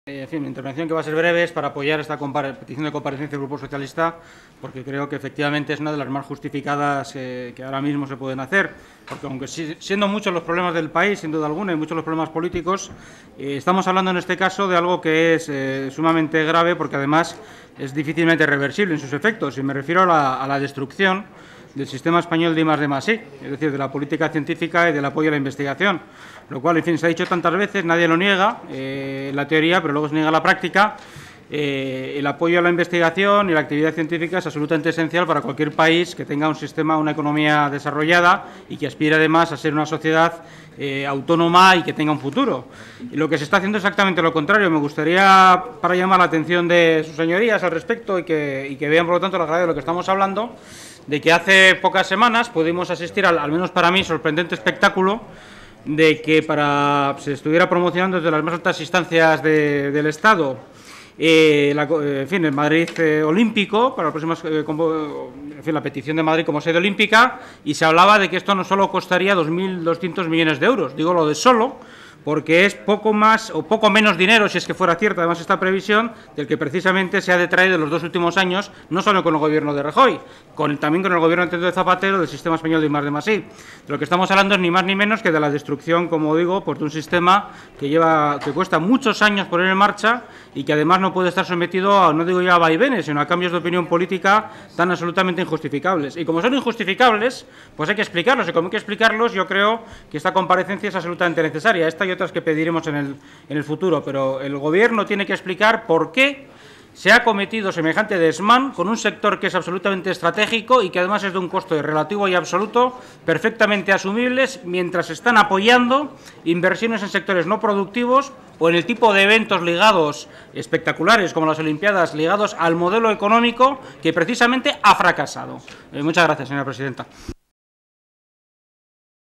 Gorriarán: "El apoyo a la investigación y a la actividad científica es absolutamente esencial"
Intervención de Carlos Martínez Gorriarán, UPyD, sobre la petición, formulada por un número suficiente de Diputados miembros de la Diputación Permanente, pertenecientes al Grupo Parlamentario Socialista, de que se acuerde la celebración de una sesión extraordinaria de la Comisión de Economía y Competitividad, con el siguiente orden del día: - Comparecencia del Ministro de Economía y Competitividad, para informar de las intenciones del Gobierno respecto de la financiación del Sistema español de I+D+i, a la vista de la situación de emergencia del Centro Superior de Investigaciones Científicas y de las previsiones de congelación de la inversión en los próximos años.

Diputación Permanente 24/07/2013